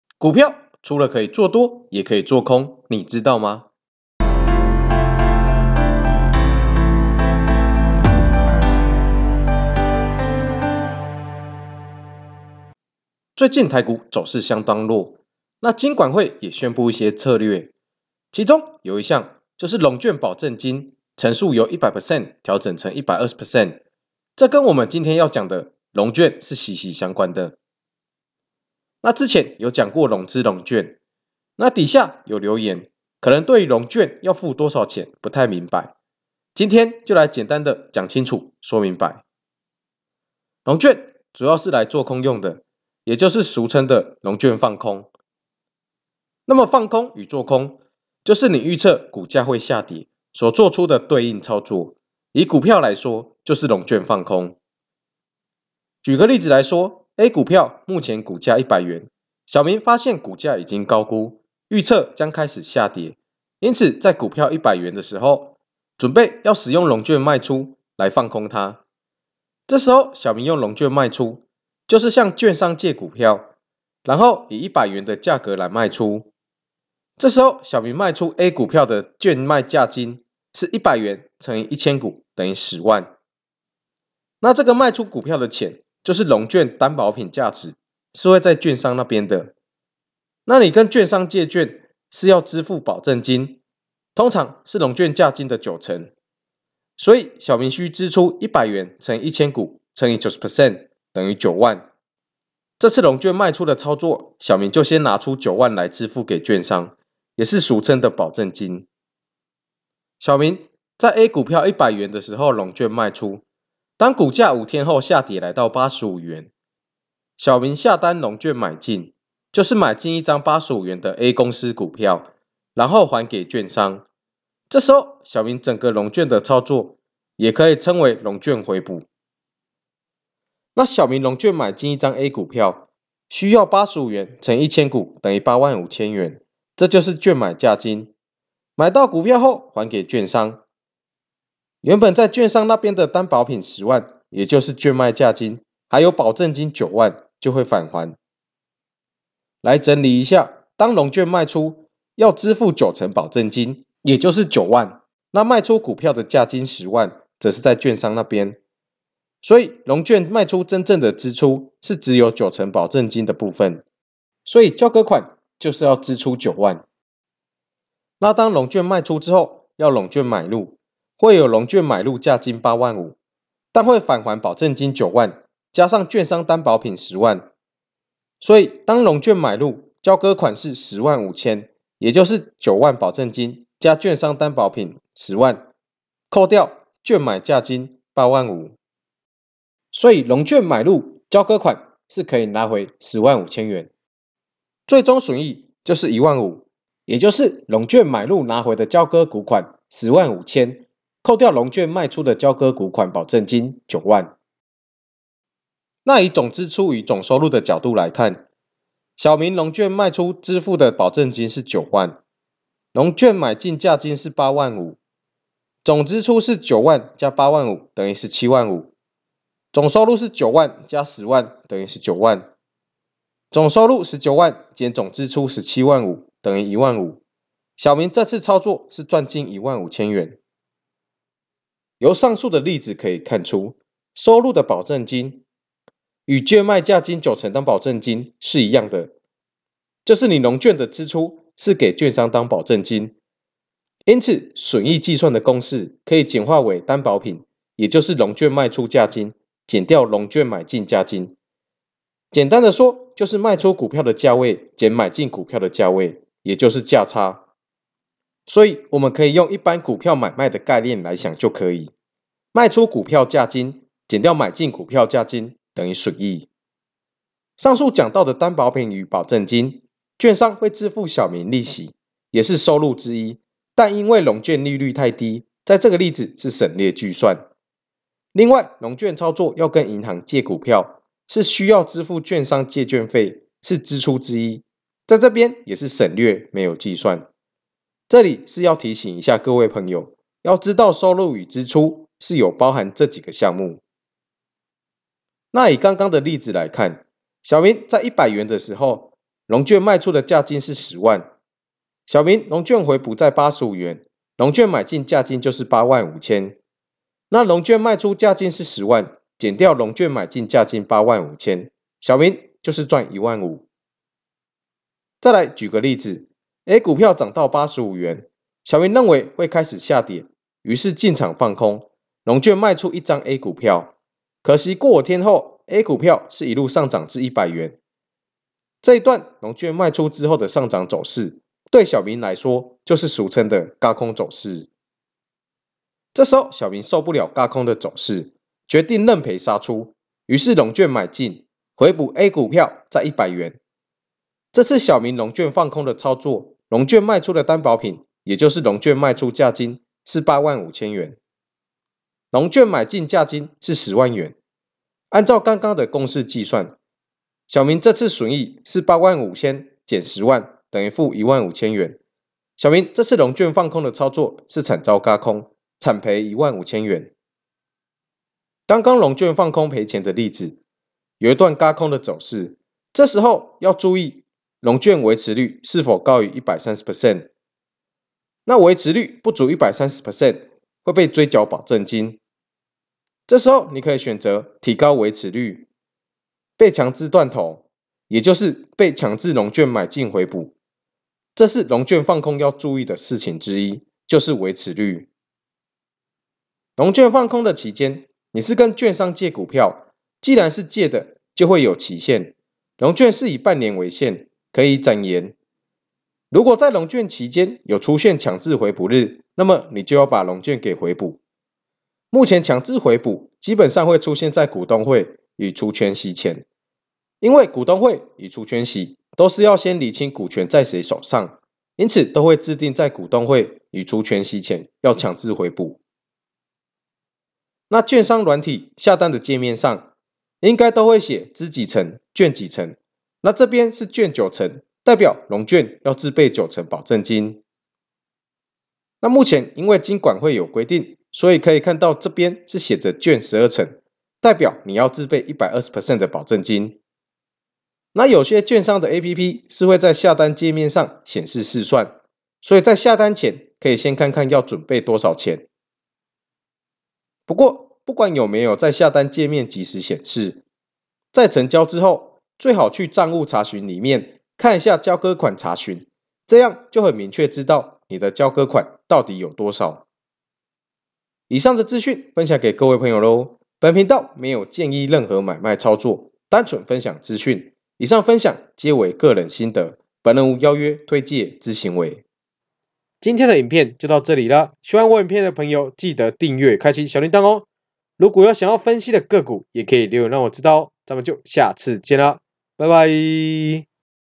股票除了作多也可以做空你知道嗎！？融券放空你一定要知道的事！！！
最近台股跌跌不休，金管會也有一些相關的策略
像是提高融券保證金成數

那麼你知道什麼是融券嗎！？
你知道維持率嗎！？
融券要注意的事情有哪些呢！？
就讓我們繼續看下去！！！